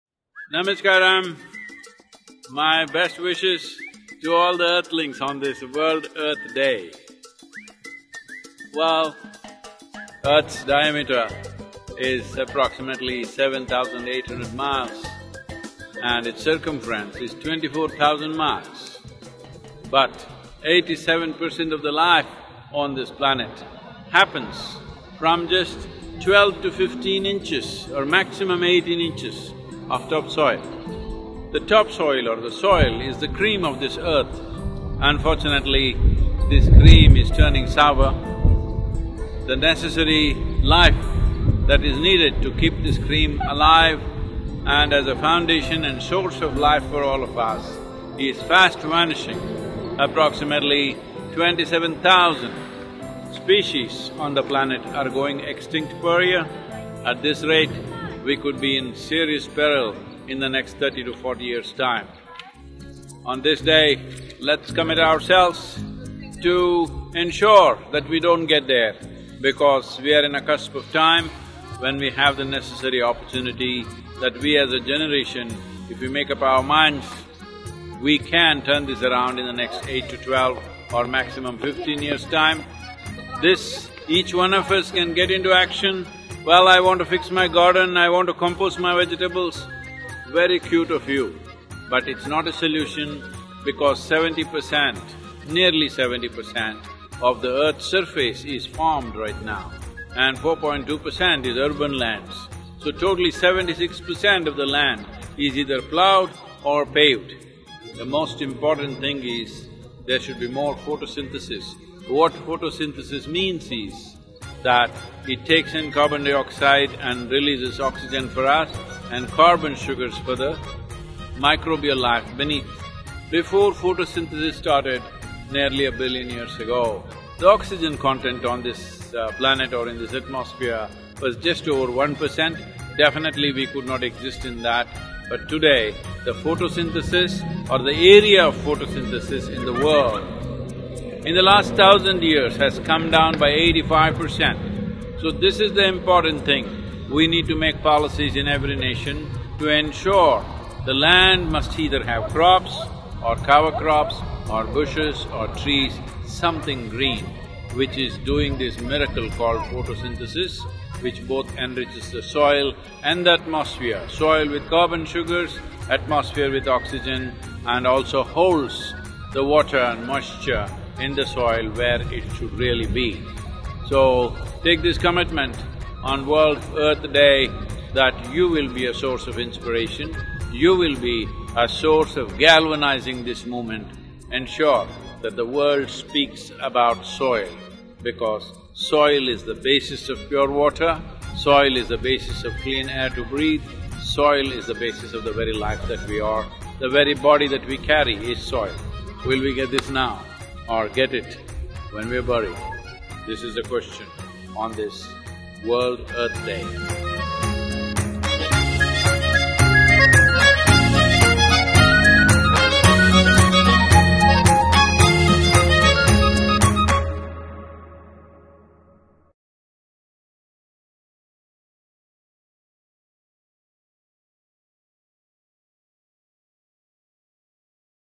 The Most Important Thing to Do On Earth Day | Sadhguru
On this Earth Day, let us commit to bringing the concern for Soil into the heart of every human being. Save Soil! Let us make it happen!